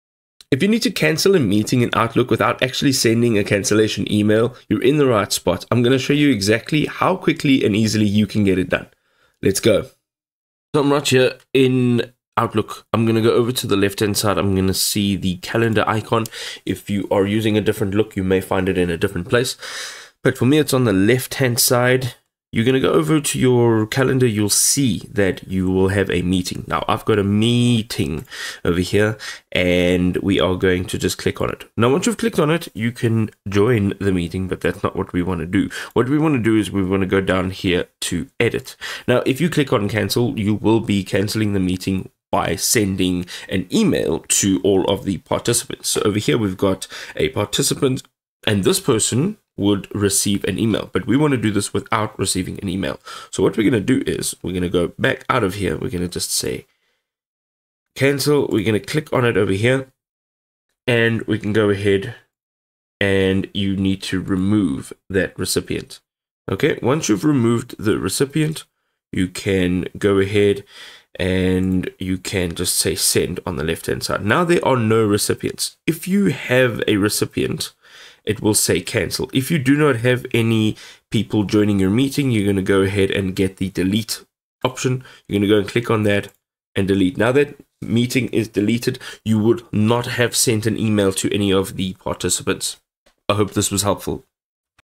How to Cancel Meeting in Outlook Without Sending Cancellation | Outlook Tutorial 2025
In this video, "How to Cancel Meeting in Outlook Without Sending Cancellation," learn the subtle yet effective method to cancel meetings discreetly without notifying attendees. This is useful when you want to remove a meeting from your calendar without creating confusion or unnecessary notifications.

We will show you step-by-step how to delete meetings from your calendar for yourself only, without sending cancellation emails to the invitees. Learn the difference between deleting meetings as an organizer versus a participant and how to manage permissions accordingly.

Additionally, discover tips for communicating cancellation separately if needed and how to handle recurring meetings carefully. This tutorial helps you maintain professionalism and avoid disruption while managing your calendar effectively in Outlook.